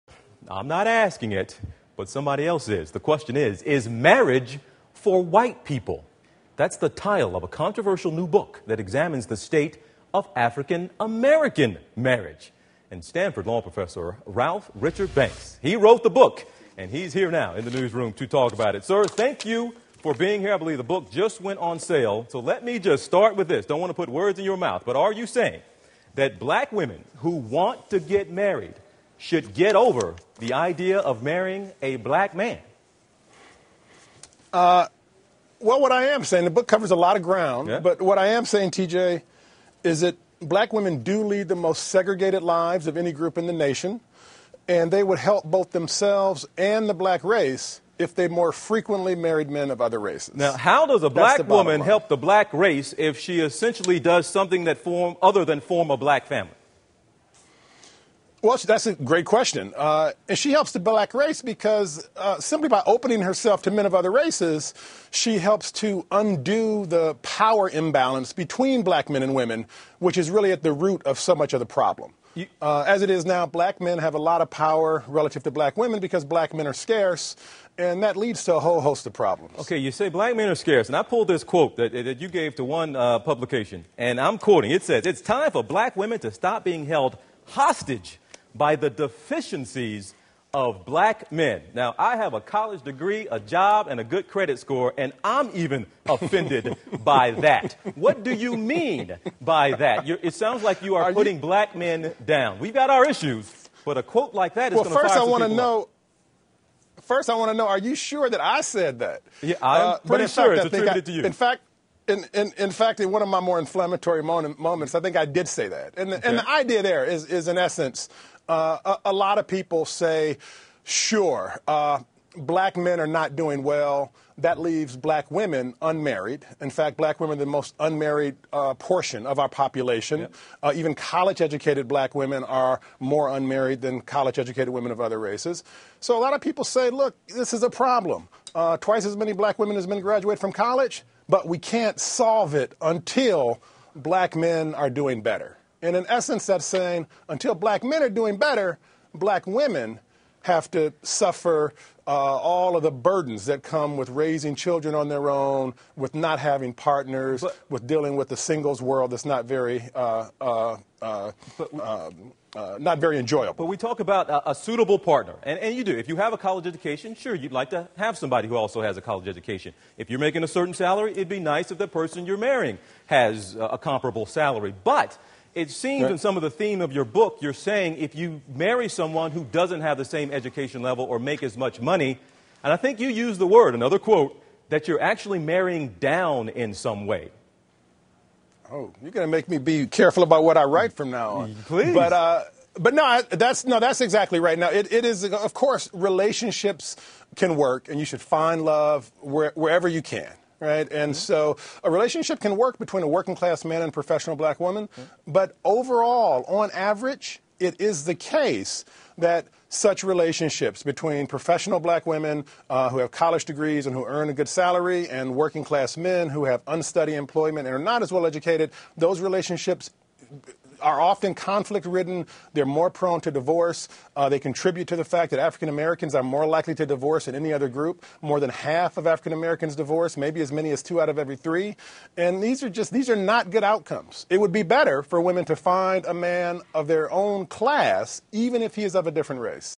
Author: 'Is Marriage for White People?'
An author says black women would help themselves and their race if they more frequently married outside of their race.